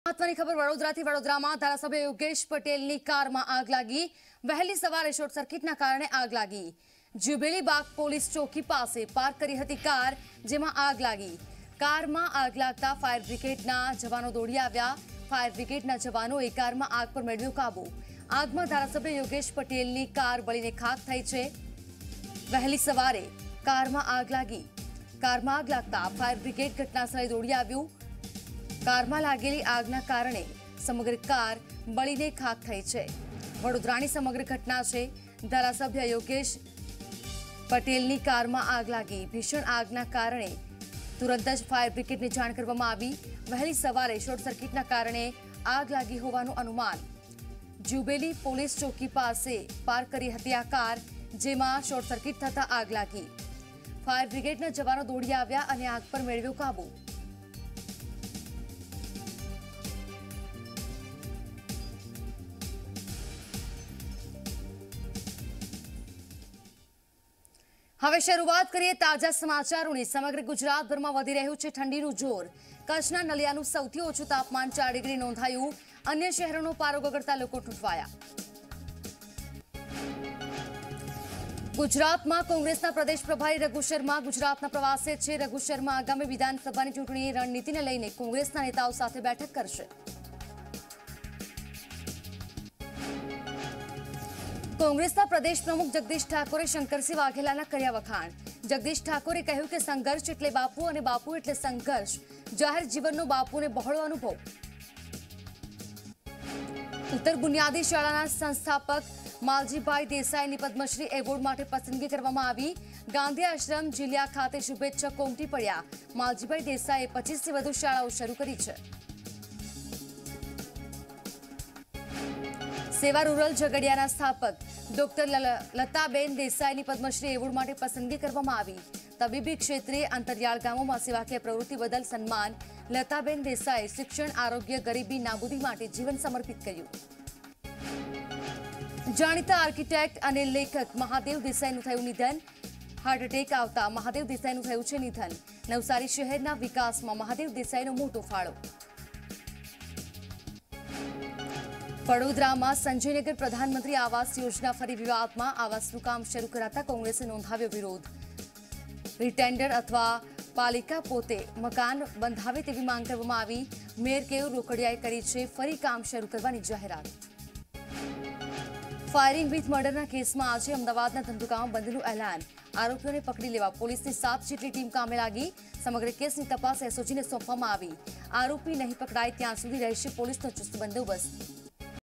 Vadodara | ધારાસભ્ય યોગેશ પટેલની કારમાં લાગી આગ | MLA | Yogesh Patel | Car Fire | Gujarati News
વડોદરામાં ધારાસભ્ય યોગેશ પટેલની કારમાં લાગી આગ--
વહેલી સવારે શોર્ટ સર્કિટના કારણે કારમાં લાગી આગ--
જ્યુબેલી બાગ પોલીસ ચોકી પાસે પાર્ક કરી હતી કાર --
કારમાં આગ લાગતાં ફાયર બ્રિગેડના જવાનો દોડી આવ્યા--
ફાયર બ્રિગેડના જવાનોએ કારમાં આગ પર મેળવ્યો કાબુ --
આગમાં ધારાસભ્ય યોગેશ પટેલનીની કાર બળીને ખાખ--